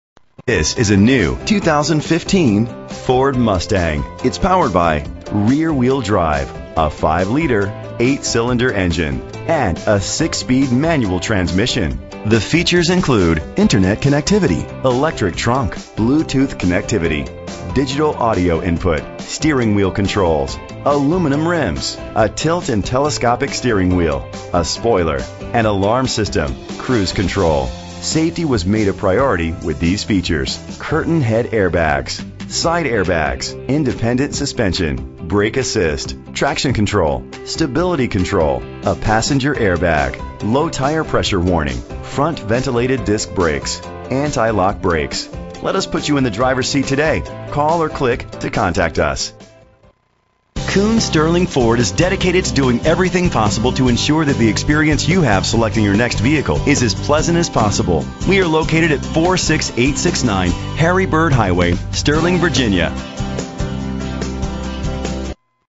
2015 Ford Mustang - Sterling VA
Phone:
Year: 2015
Make: Ford
Model: Mustang
Trim: GT
Engine: 5.0 liter 8 cylinder 32 valve
Transmission: 6-Speed Manual
Color: Race Red
Mileage: 0
Address:
46869 Harry Byrd Highway